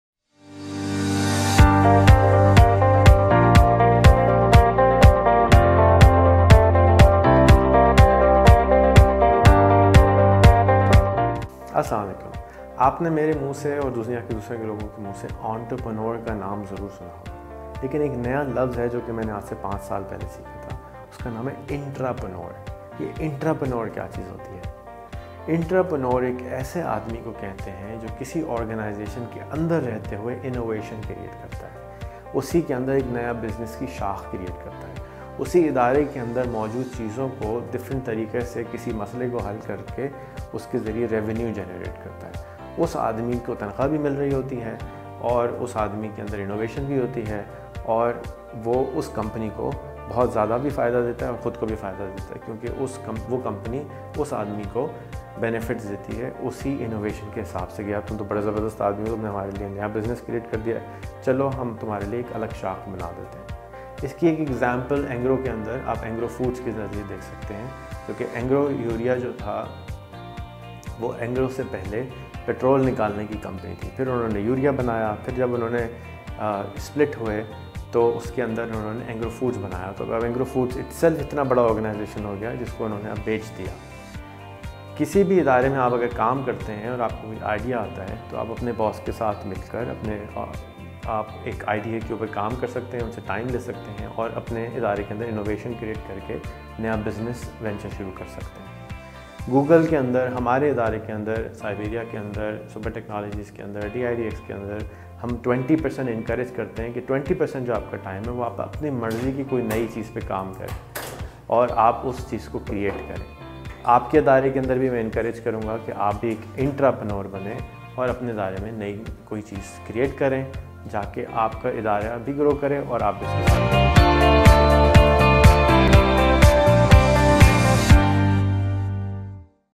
Intrapreneur Kiya hai | Rehan Allahwala | entrepreneur | Intrapreneurship
a manager within a company who promotes innovative product development and marketing.